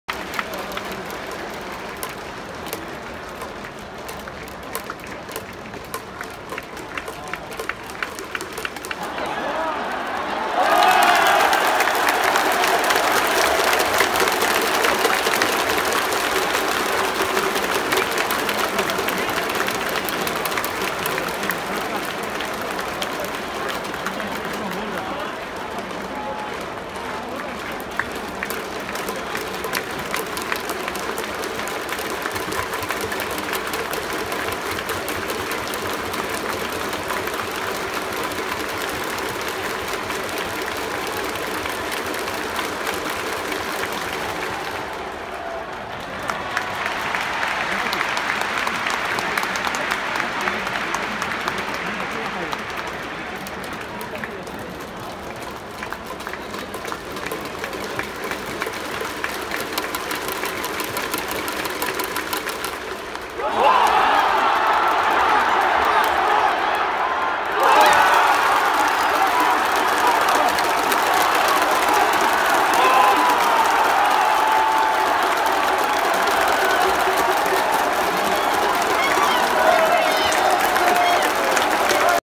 【20210924】公式戦 讀賣 - 阪神 東京ドーム【9回表 サンズ】
阪神
9回表
サンズ
無死1塁
1塁 糸原 →代走: 植田
一塁走者 植田 :積極的に初球から走って盗塁成功 2塁
ランナー2塁の1-1からセンターへの同点タイムリーツーベース！ 巨6-6神 2塁